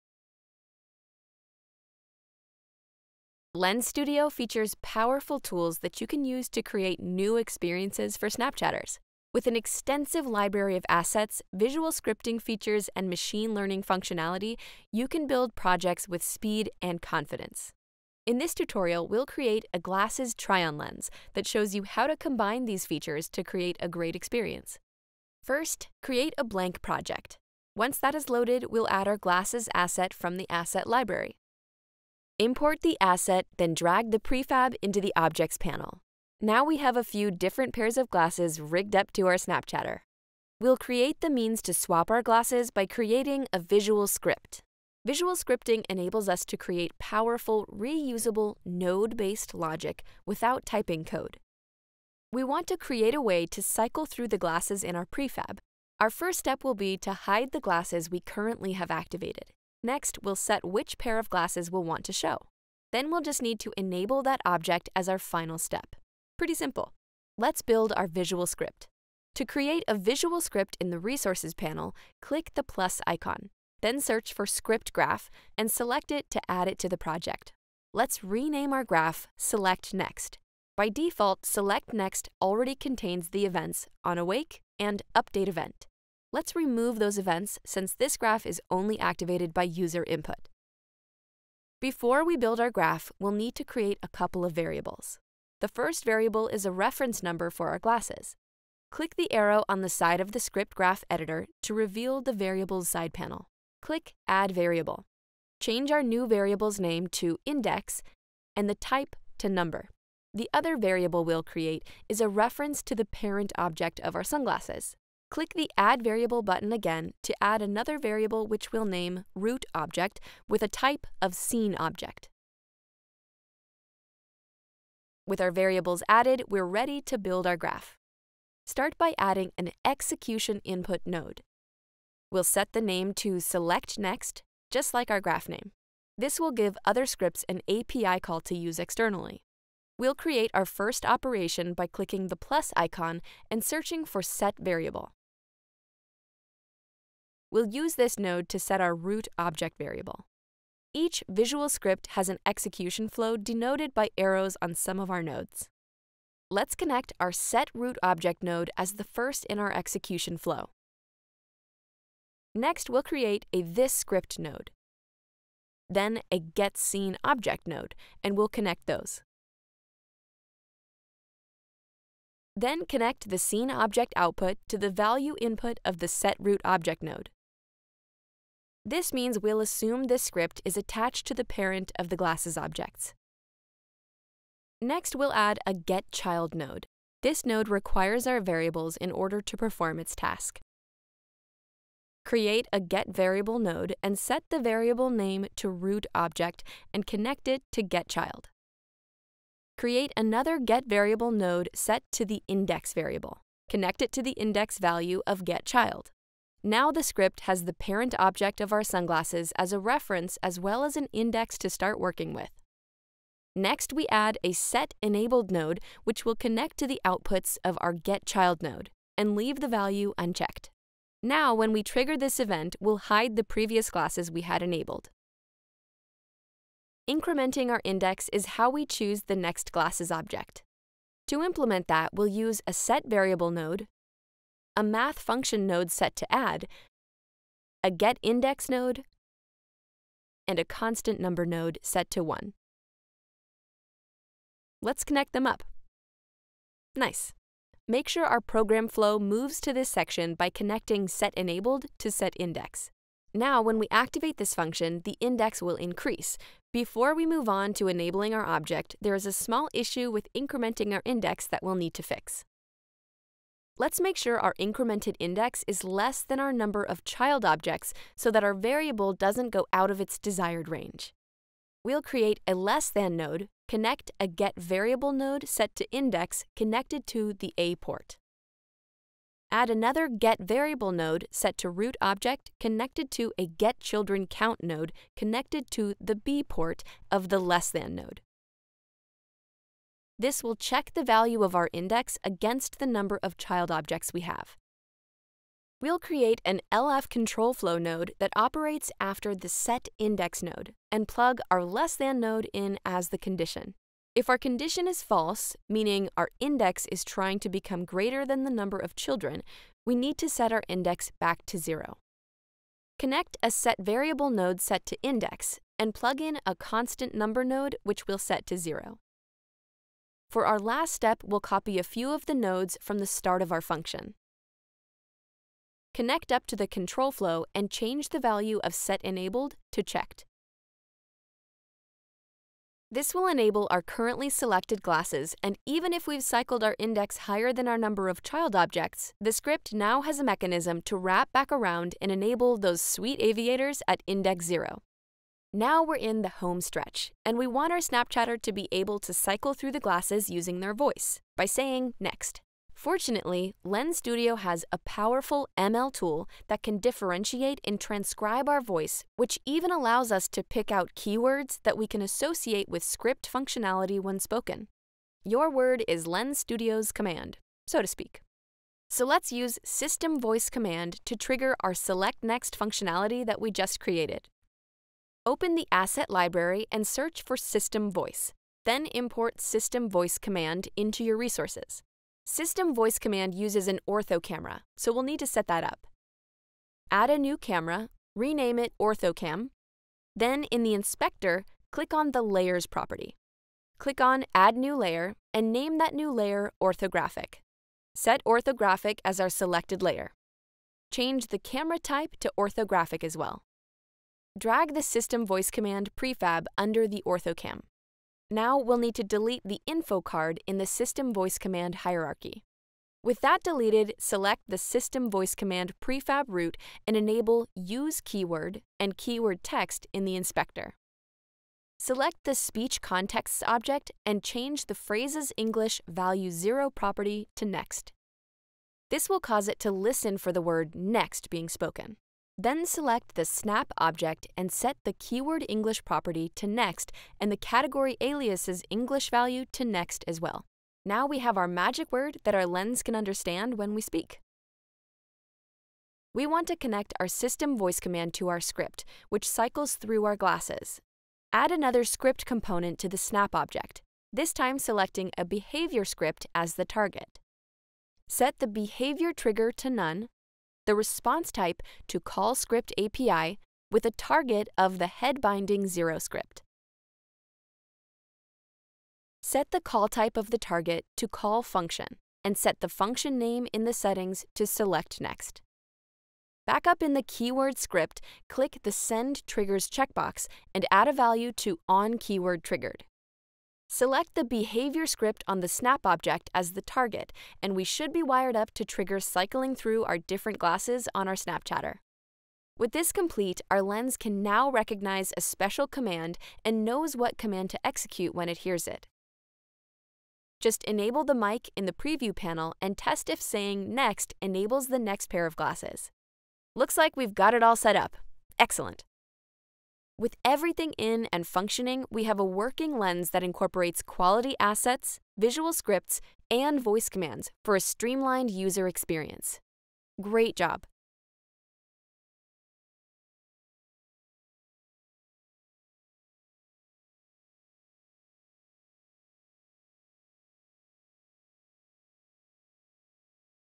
How to Make AR Glasses Try-on Lens with Visual Scripting in Lens Studio
In this Lens Studio tutorial you will learn how to create an AR Glasses Try-on Lens for Snapchat with a carousel that's powered by a Visual Scripting graph.

00:00 - Introduction
00:27 - Creating blank project
00:30 - Importing Assets from Asset Library
00:45 - Visual Scripting Introduction
01:10- Creating Visual Script Graph
01:35 - Creating variables
01:43 - Creating Variables
02:10 - Adding Execution Input Node
02:25- Adding Set Variable
02:40 - Connecting nodes
02:45 - Creating This Script and getSceneObject nodes
02:56 - Connecting gestSceneObject to Set rootObject node
03:05 - Adding getChild node
03:15- Creating getVariabel node, set to index
03:35 - Adding setEnabled node
03:48 - Incrementing Index to choose next object
04:30 - Setting up variable range
05:00 - Understanding conditions
05:50 - Adding voice controls
06:10- Adding System Voice Command
06:18 - Importing System Voice Command from Asset Library
06:25 - Adding new Orthographic Camera
06:45 - Adding SystemVoiceCommand to Ortho Cam
06:55- Setting up SystemVoiceCommand
07:01- Enabling Use Keyword and Keyword Text
07:07- Setting up SpeechContexts
07:15- Setting up KeywordParent
07:25 - Connecting SystemVoiceCommand with Visual Graph script
08:20 - Testing Voice Command